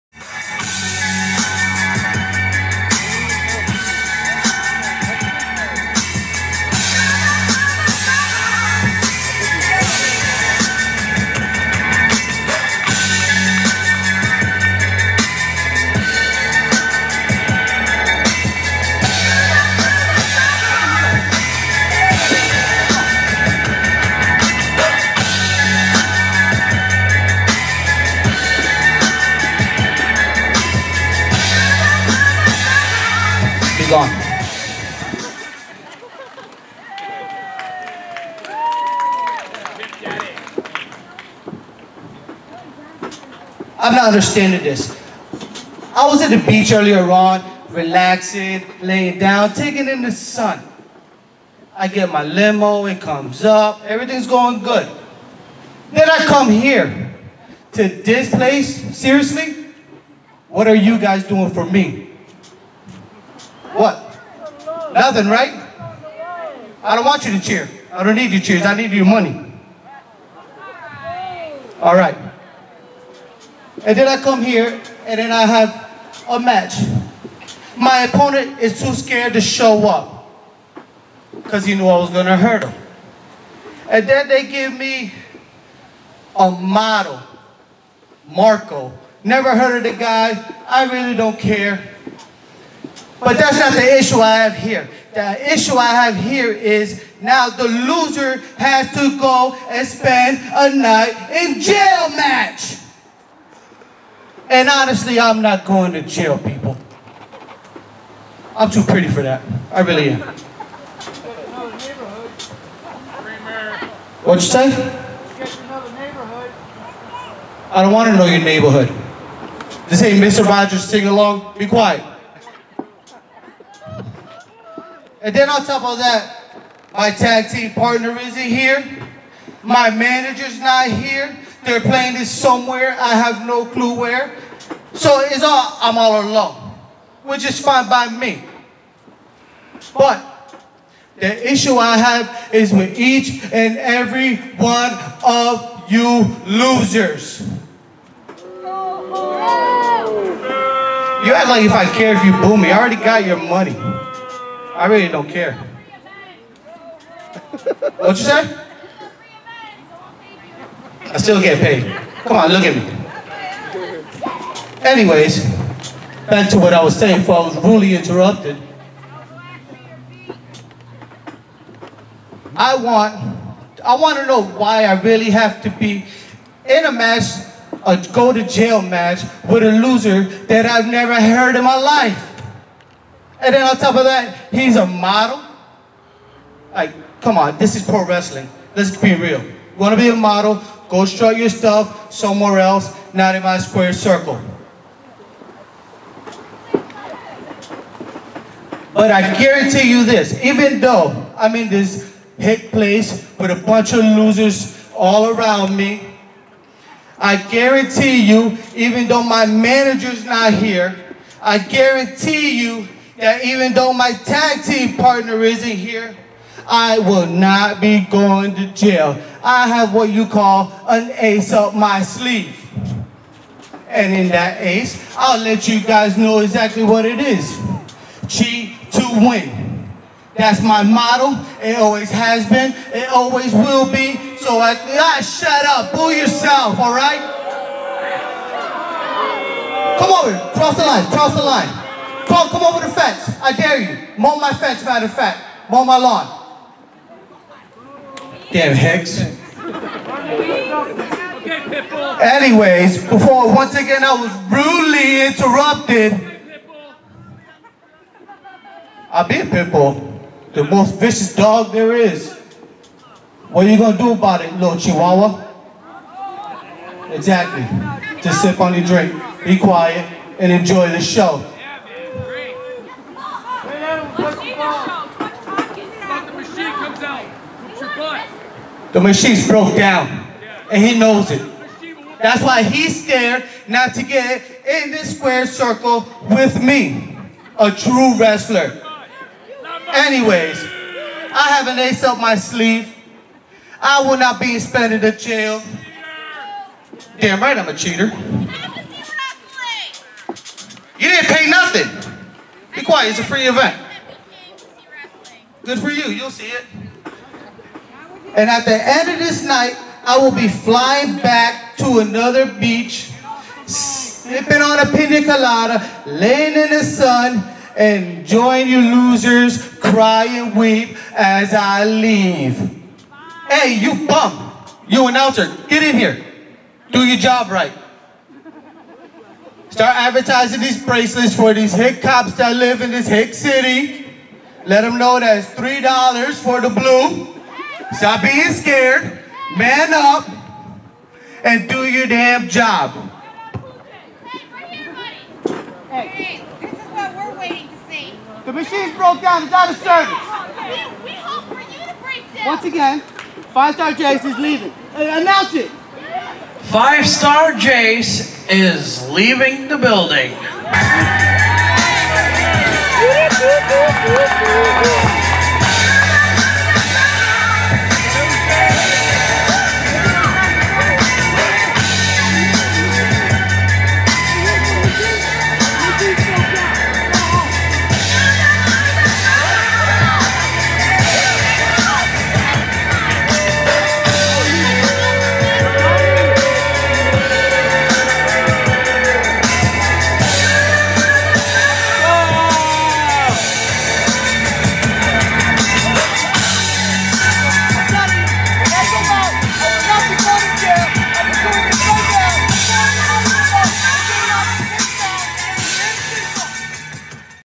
NAOTW - 8.20.16 - Five Star Jase speaks on his "Loser Goes to Jail" Match
New Age Old Tyme Wrestling - 8.20.16 - Five Star Jase speaks on his "Loser Goes to Jail" Match